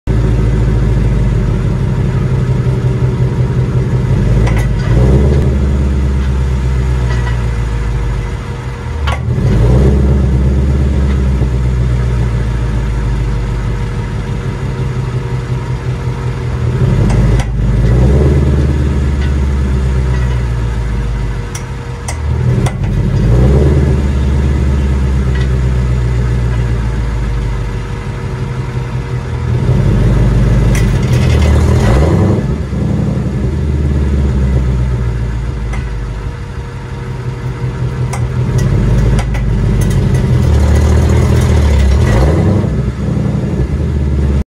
Diff crunch
Crunchy rear diff in 2015 Mustang GT

Factory 8.8 3.73 and Torsen